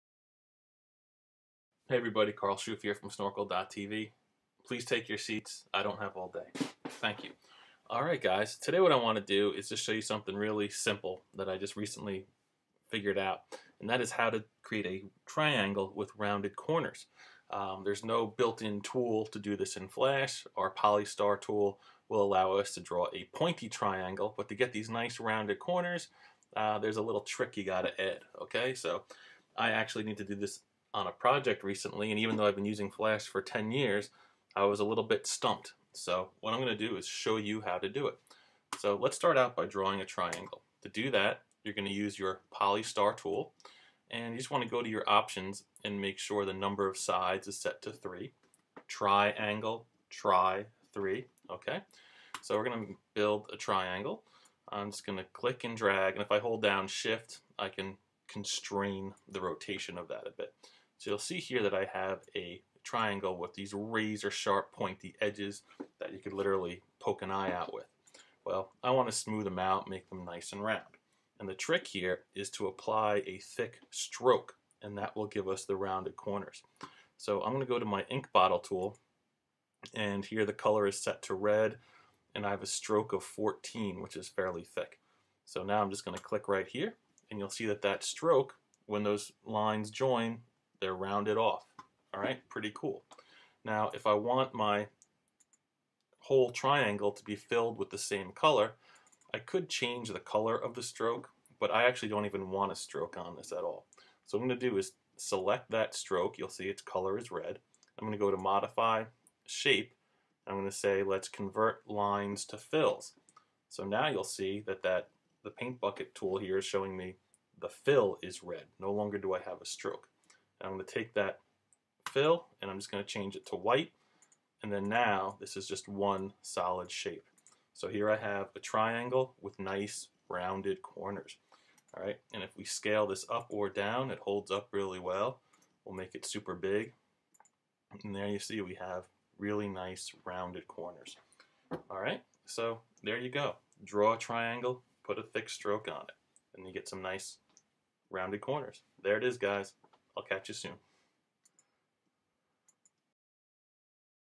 Create A Triangle with Rounded Corners with Flash
neat little tip for anyone. very easy. just put a thin stroke on the triangle!